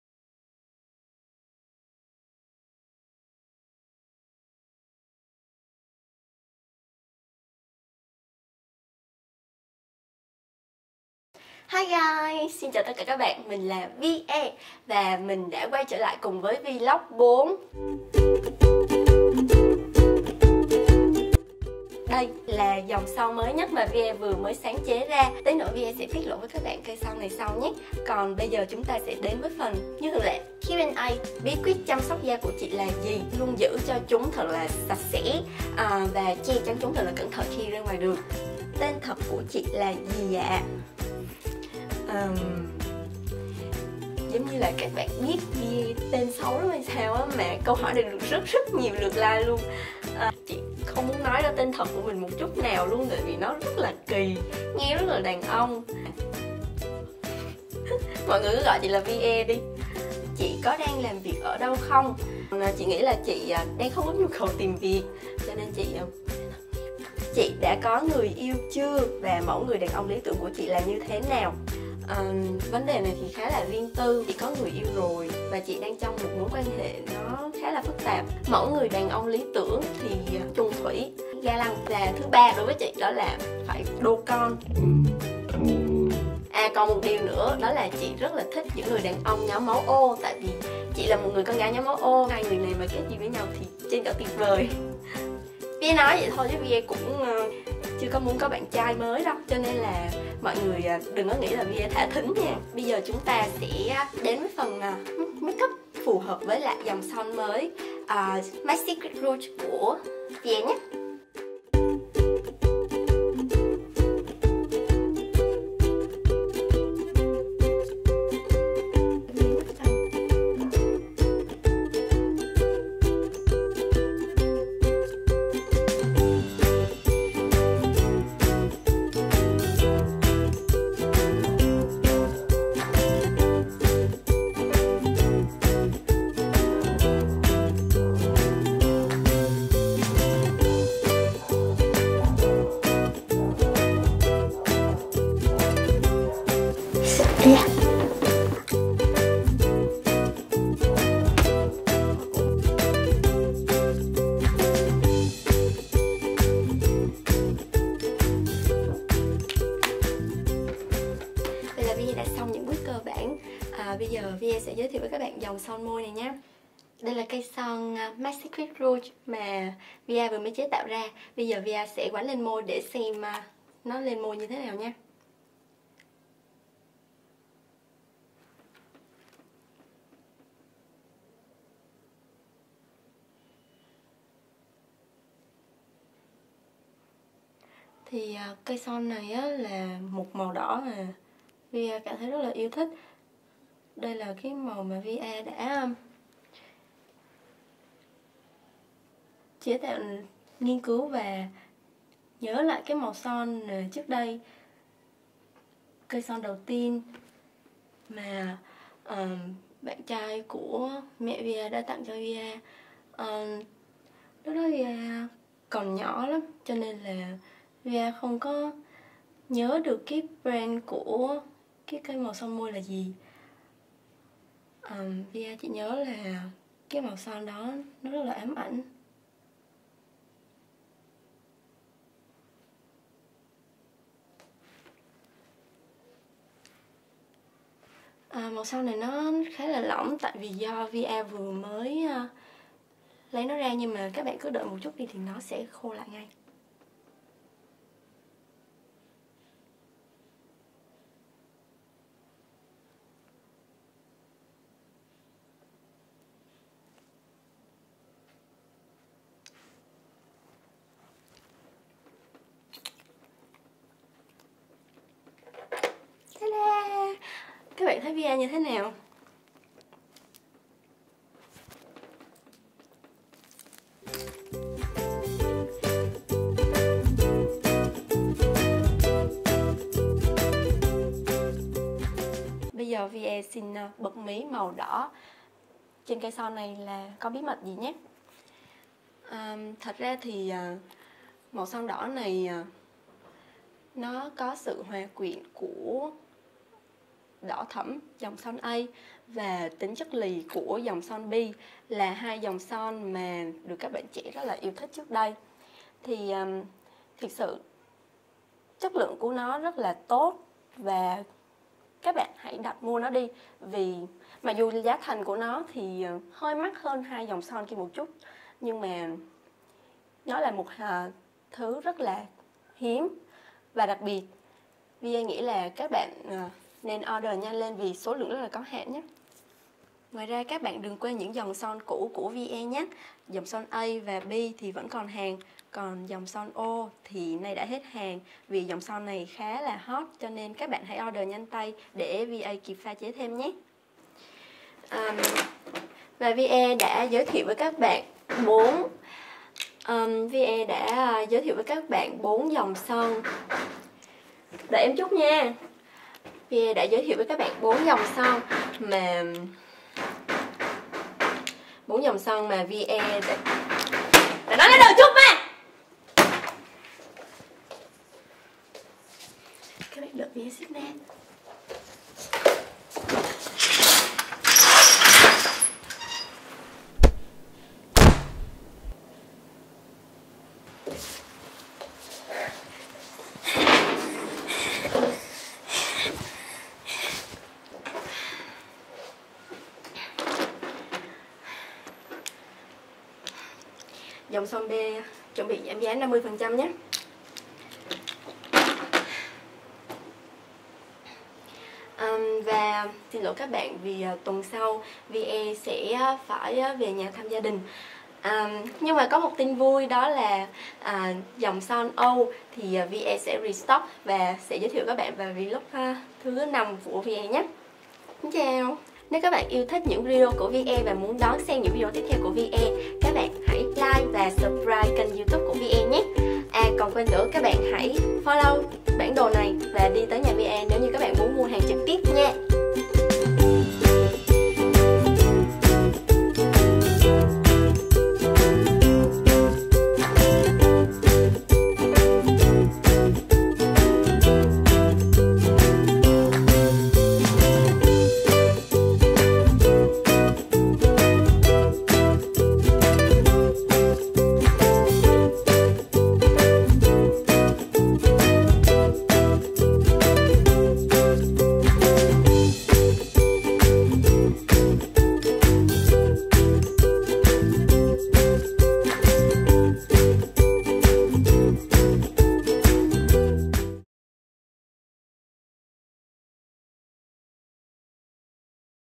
VLOG4: MY SECRET ROUGE - 48HFP - JUNETEAMSIX
We are fun makers making film(s)

- This film was made for 48 hours film project -
48 HOURS FILM PROJECT 2017 - DỰ ÁN LÀM PHIM TRONG 48 GIỜ
Tên nhân vật: Lê Minh Viên
Đạo cụ bắt buộc: Bản đồ
Câu thoại bắt buộc: Thả thính à?
Thể loại: Adventure or Mockumentary
Diễn viên:
Vũ Thị Thu Giang     -        Vie/Lê Minh Viên
Linh Khánh Nguyễn -        Xác chết 1
Lê Trường                 -        Xác chết 2
Hiếu Già                    -         Giám đốc sản xuất
Trần Thành               -         Đạo diễn
                                              Phó đạo diễn
                                              Dựng phim
                                              Mua nhạc nền
                                              Phó quay phim
Hồ Hải Quỳnh           -         Biên kịch
                                             Thiết kế máu
                                              Chỉ đạo diễn xuất
                                              Chủ nhiệm
Min Jun                     -         Sản xuất
                                              Cố vấn nghệ thuật
                                              Best girl
                                              Ăn uống hiện trường
                                              Dựng phim
Hiếu Nghiêm            -          Đạo diễn hình ảnh
                                              Chỉ đạo ánh sáng
                                              Quay phim
                                              Dựng phim
                                              Kỹ thuật màu phim
                                              Kỹ xảo điện ảnh
                                              Thiết kế âm thanh
Trường Lê                 -          Họa sĩ thiết kế
                                              Nghệ sĩ kỹ xảo
                                              Trợ lý phó đạo diễn
                                               Lá Me
Linh Khánh Nguyễn -          Quay phim
                                               Kỹ thuật VTR
                                               Best Boy
                                               Ánh sáng
                                               Cầm boom
                                               Vận tải
An Du                        -           Hỗ trợ thiết bị
Bối cảnh quay          -           Chaosdowntown
Huỳnh Hoàng Phi/   -           Âm thanh
Phan Phú Cường

Đây là trải nghiệm điện ảnh sướng nhất từ trước tới giờ của tụi mình. Cảm ơn thế giới!